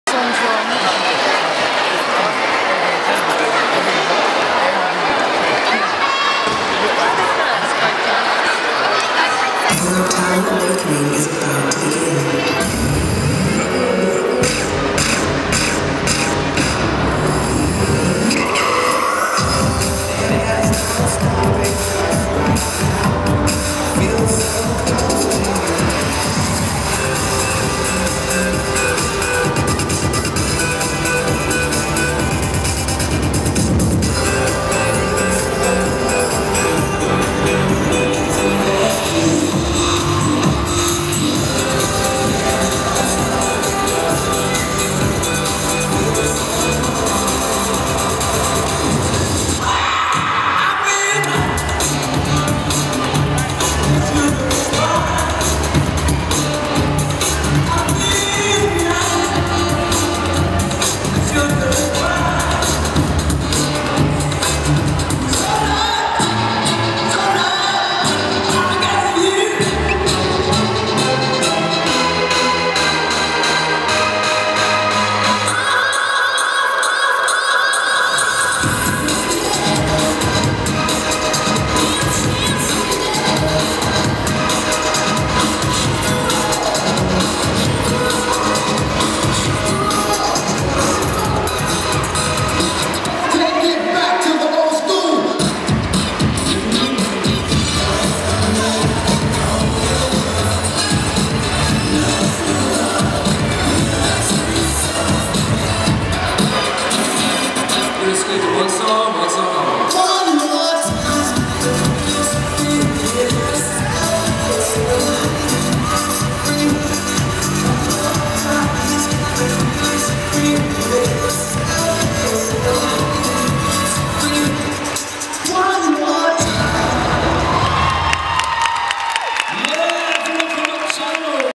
Nocturnal - Essex Championships 2013
Under 16 Beginners - Nocturnal - 3rd Place

101 Events Company

Essex Street Dance Championships 2013

Bpm Dance Academy

*This Track is Used for Inspirational Purposes Only!*